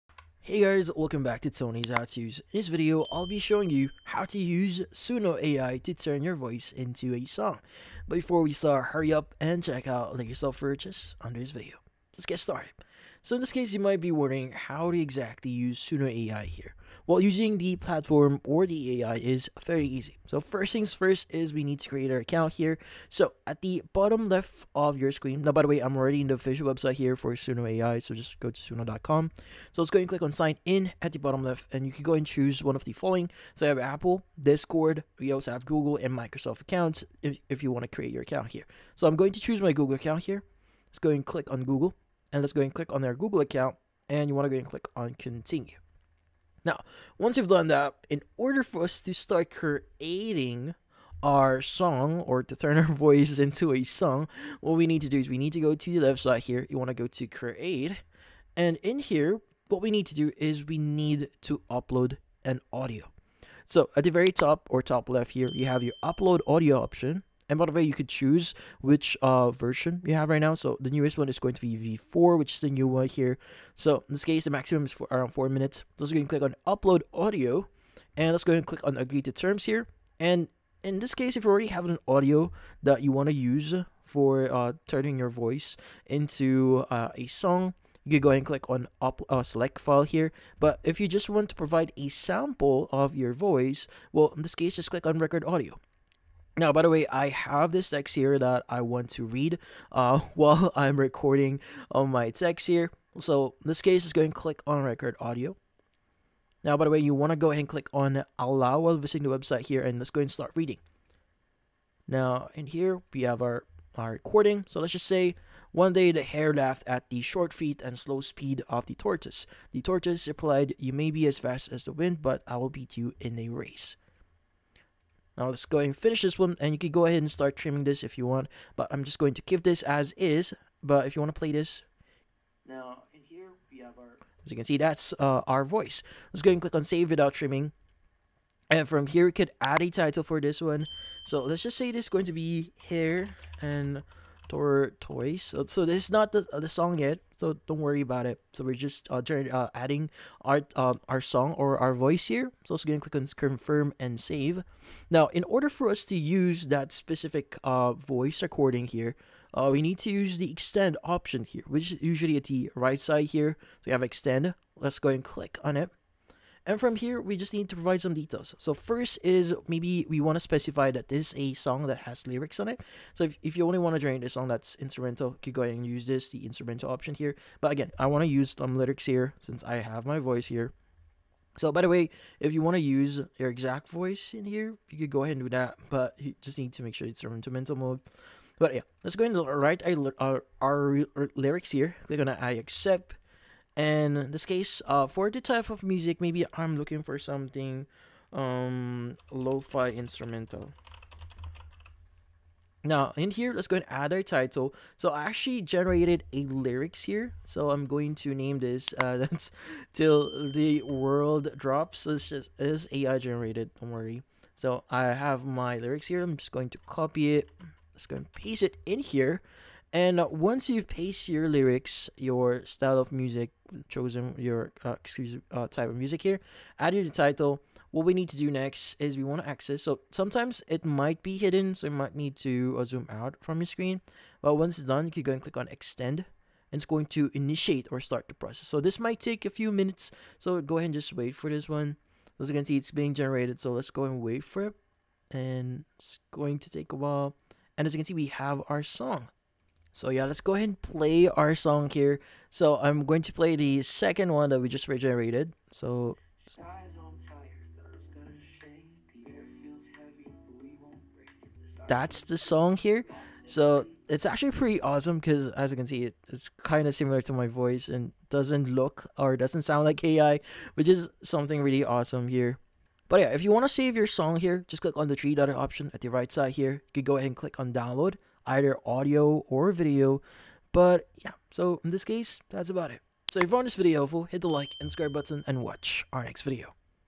How to Use Suno AI to Turn Your Voice Into a Song (2026)
In this video, I show you How to Use Suno AI to Turn Your Voice Into a Song in 2026. The quick and easy tutorial how to use suno ai with my voice properly. Do you want to know step by step how suno ai works with your voice in 2026? Yes? Great!